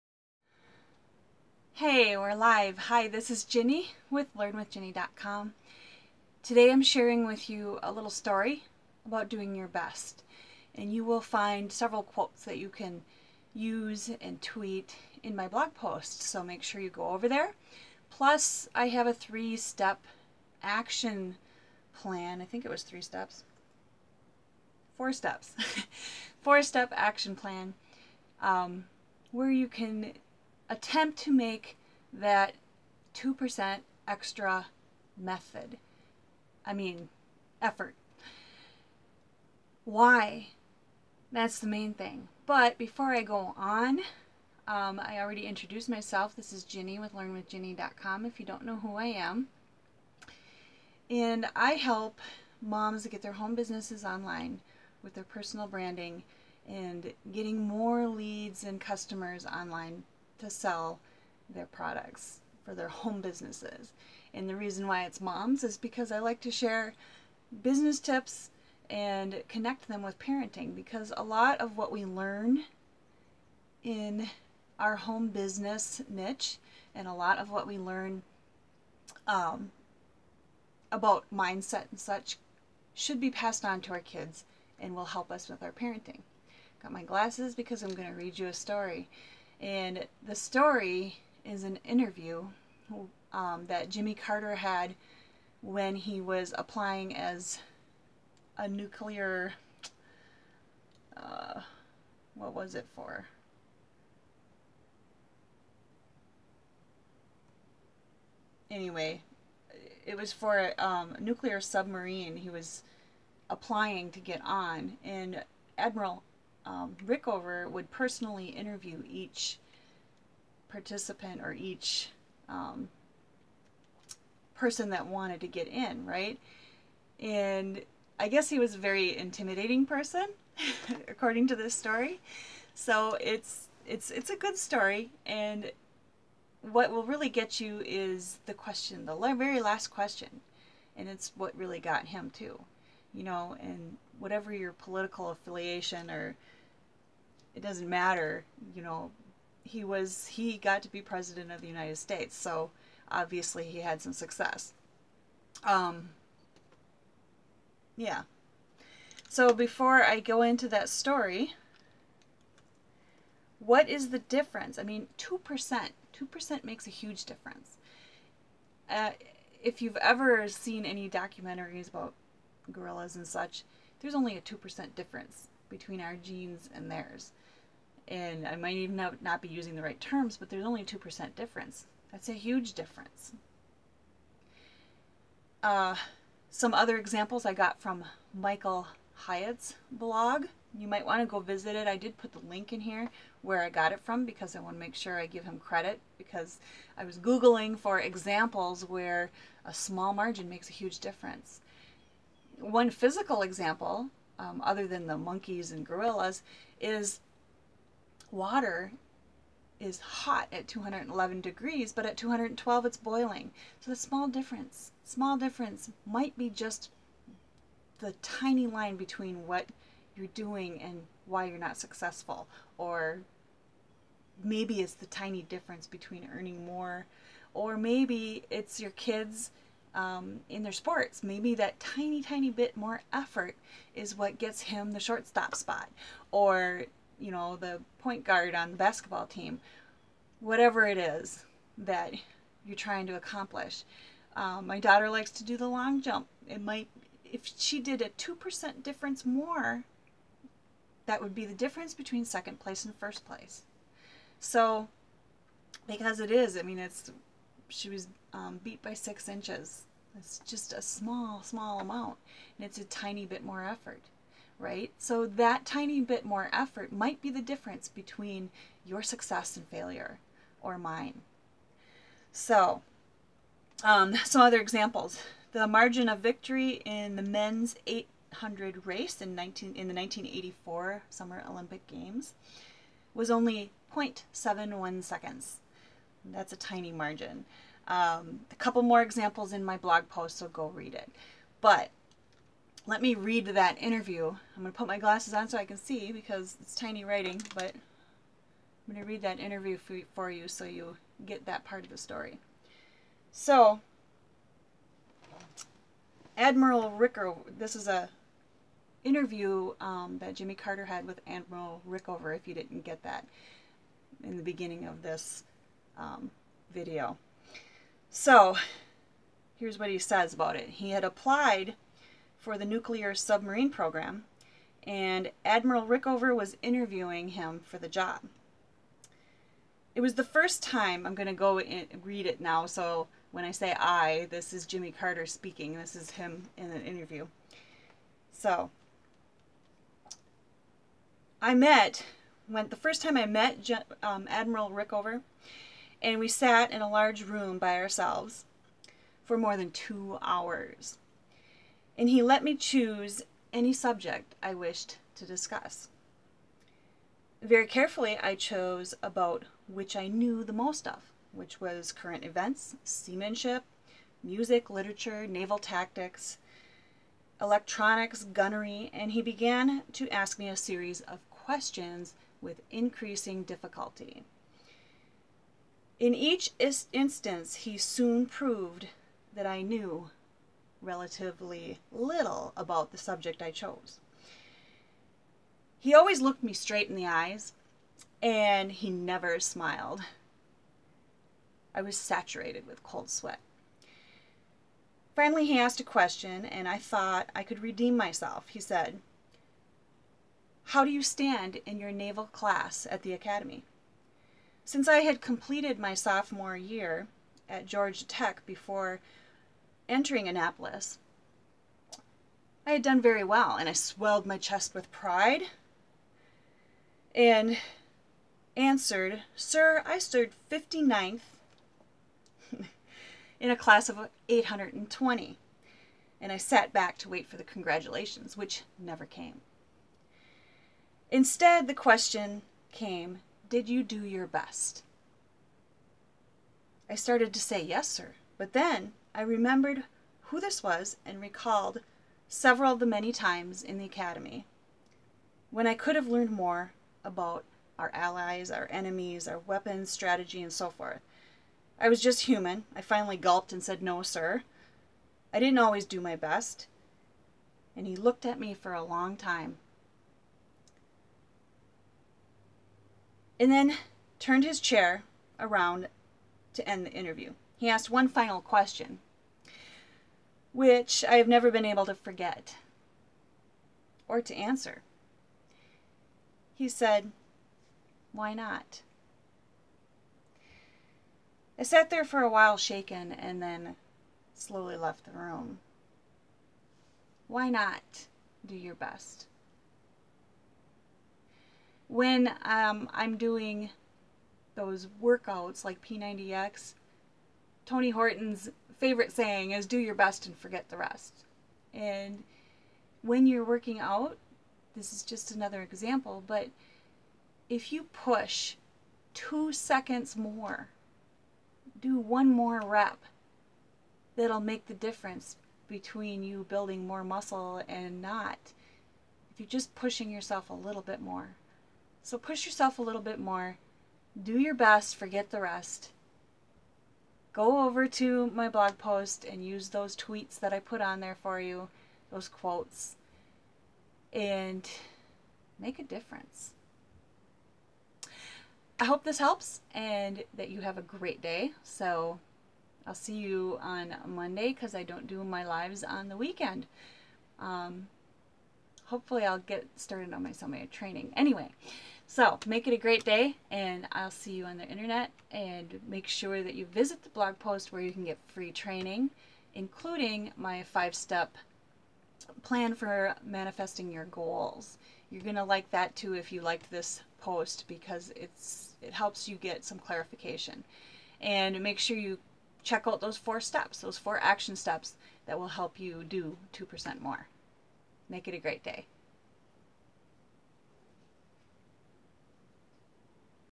Do Your Best
A story - of an interview between Jimmy Carter and Admiral Rickover.  Doing your best and doing just 2% better or more - might be the only thing lying between you and success.